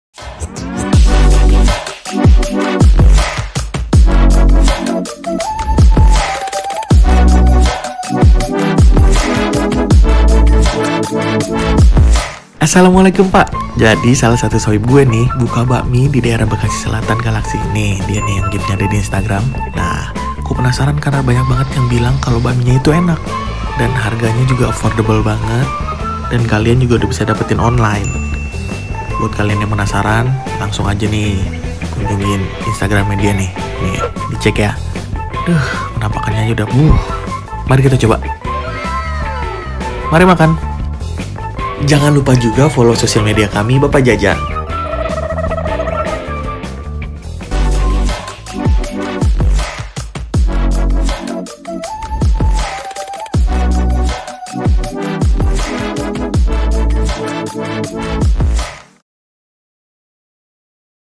Bakmie Bangper
Bakmie Bangka Bangper

Yoo pack mohon maap masih dalam masa recovery nih dari corona.

Kali ini makan bakmie bangka salah satu buatan sohib saya pack, rasanya enak banget apalagi kaldu nya sampe saya sruput tuh mangkok pack. sudah bisa dipesan juga melalui gofood atau grabfood dimasa ppkm ini. silahkan cek instagram nya di @bakmie.bangper